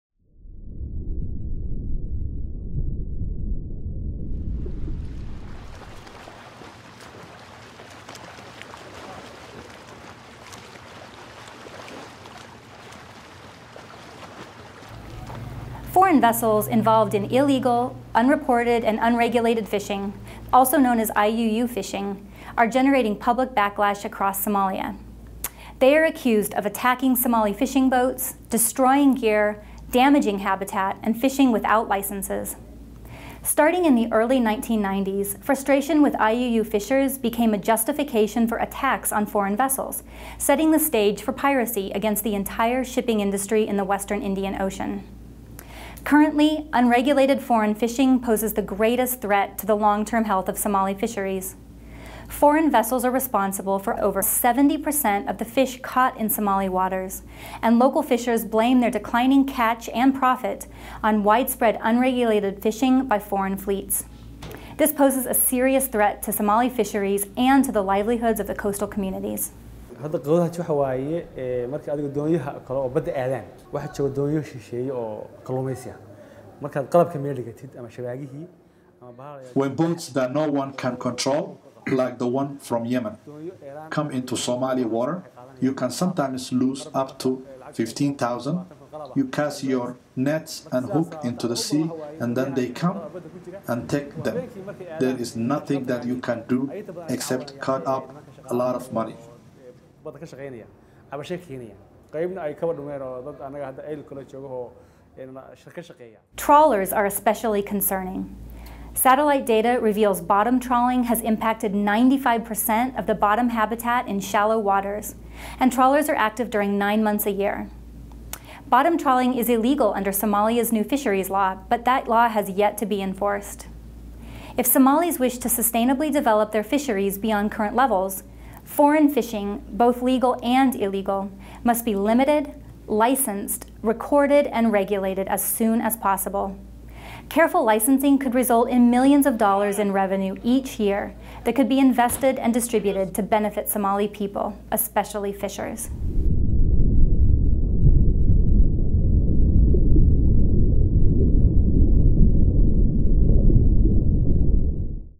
Illegal, Unreported, and Unregulated Fishing - Securing Somali Fisheries
Foreign vessels involved in illegal, unreported, and unregulated (IUU) fishing are damaging Somali fish populations, marine habitat, and the welfare of Somali fishers. Our report, Securing Somali Fisheries, finds that foreign vessels remove 77% of the total fish caught in Somali waters. Foreign trawlers are causing habitat destruction and have impacted 95% of the bottom habitat in shallow waters. This level of unregulated fishing threatens the fish stocks and the livelihoods and food security of Somalis, who lack the capacity to control these foreign vessels. For sustainable fisheries development, IUU fishing must stop, and these foreign vessels need to be limited, licensed, recorded, and regulated immediately.

Report Authors
Sarah M. Glaser
Paige M. Roberts
Robert H. Mazurek
Kaija J. Hurlburt
Liza Kane-Hartnett

Graphics
Timothy Schommer
Andrea Jovanovic

Videography
Jean-Pierre Larroque

Featured Shuraako Clients
Al Fatxi Fishing
Alla Amin Fishing
Dur Dur Fishing
Hodan Fishing
Somali Fair Fishing
Zakia Fishing

Stock Photos
Getty Images
iStock
Wikipedia:
Robert K. Brigham
Mass Communication Specialist 2nd Class Jason R. Zalasky

Secure Fisheries is a Project of the
One Earth Future Foundation